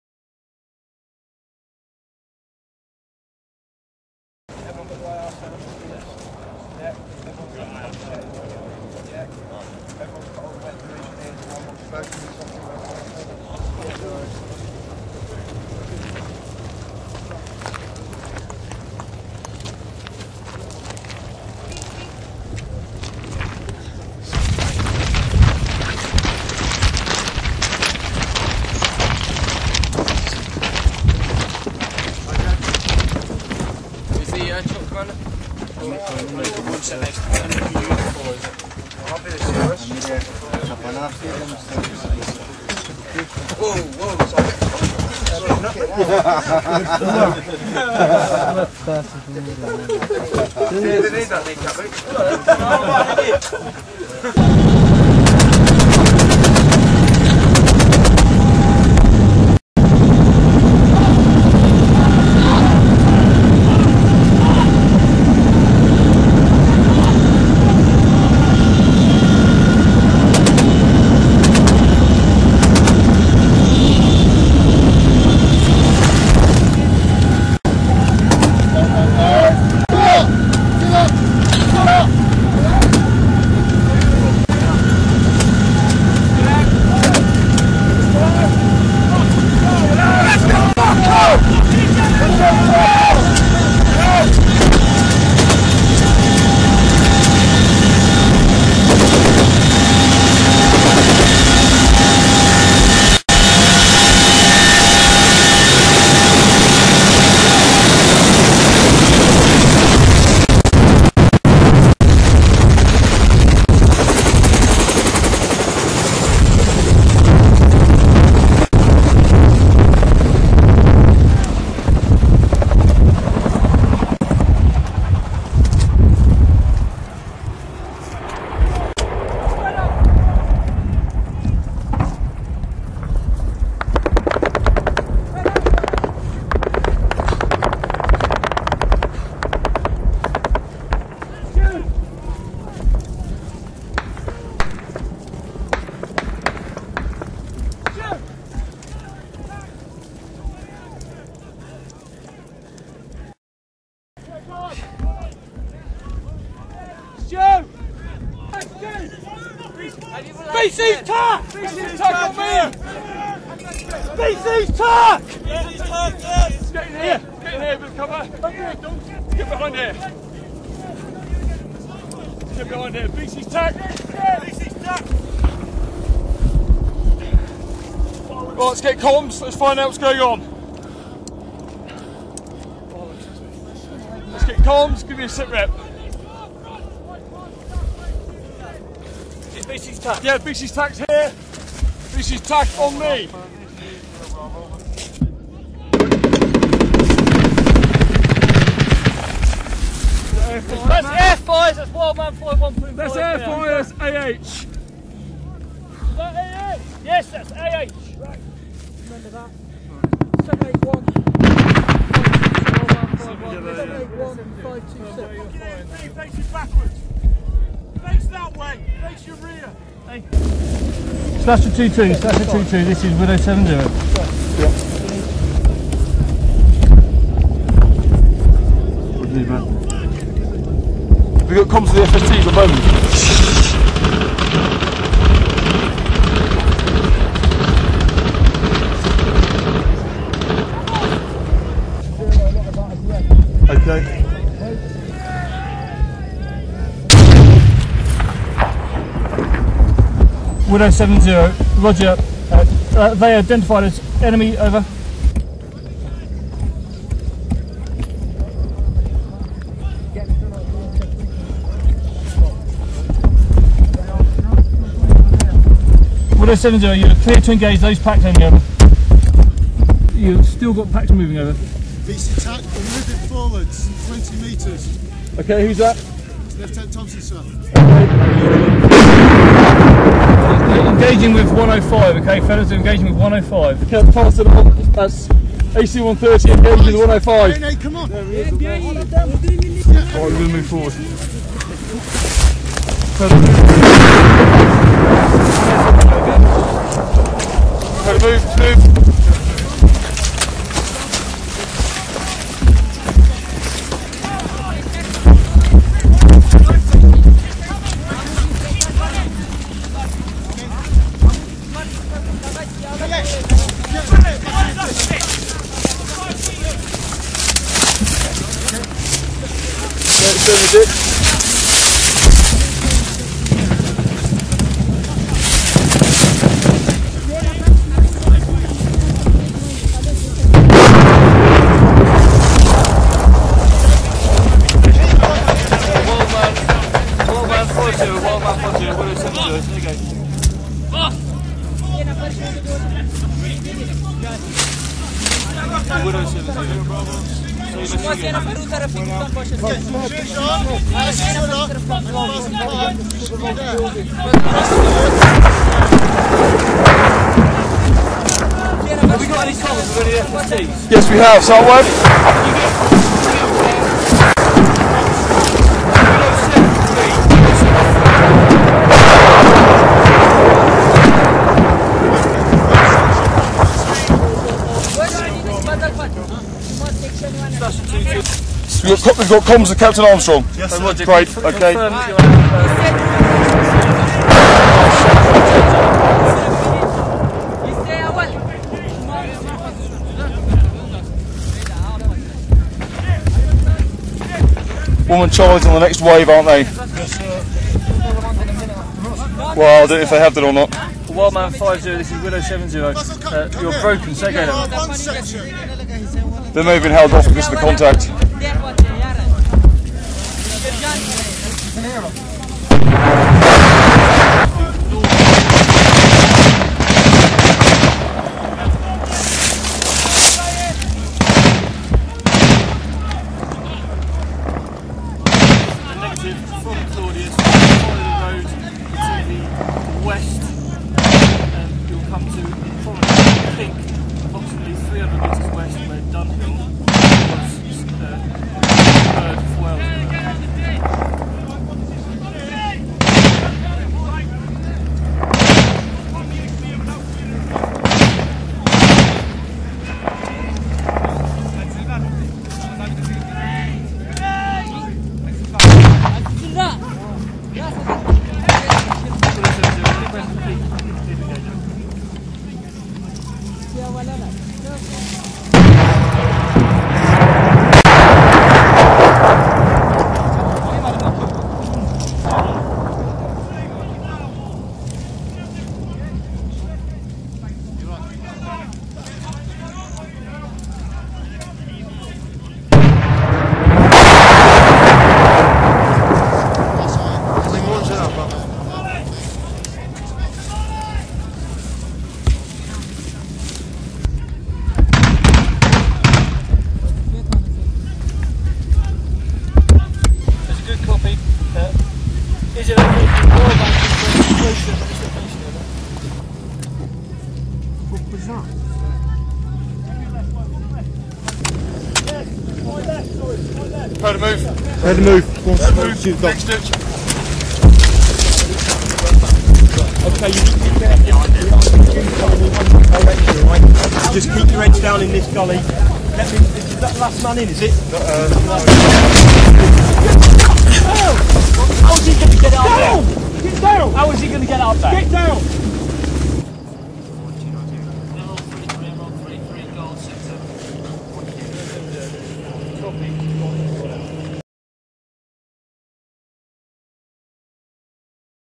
British Army -Part 1 Operation Augustus Helmand, Afghanistan
Part one of a vid about Operation Augustus In Helmand Province, Afghanistan I am sorry for bad/loud sound and quality but this is a frontline video so it isn't the best quality